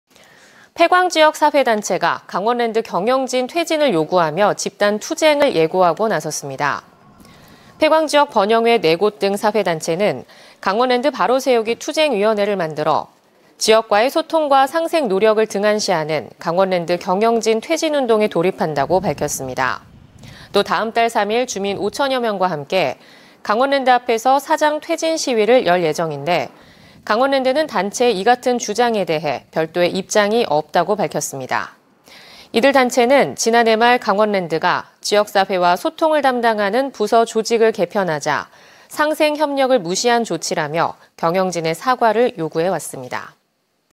[단신] 폐광지역 사회단체 '강원랜드 경영진 퇴진' 집단 투쟁 예고 220208
폐광지역 사회단체가
강원랜드 경영진 퇴진을 요구하며,
집단 투쟁을 예고하고 나섰습니다.

폐광지역 번영회 4곳 등 사회단체는
'강원랜드 바로세우기 투쟁위원회'를 만들어
지역과의 소통과 상생 노력을 등한시하는
강원랜드 경영진 퇴진 운동에 돌입한다고 밝혔습니다.

또, 다음 달 3일
주민 5천여 명과 함께 강원랜드 앞에서
사장 퇴진 시위를 열 예정인데,
강원랜드는 단체의 이 같은 주장에 대해
별도의 입장이 없다고 밝혔습니다.

이들 단체는 지난해 말,
강원랜드가 지역사회와 소통을
담당하는 부서 조직을 개편하자
상생 협력을 무시한 조치라며
경영진의 사과를 요구해 왔습니다.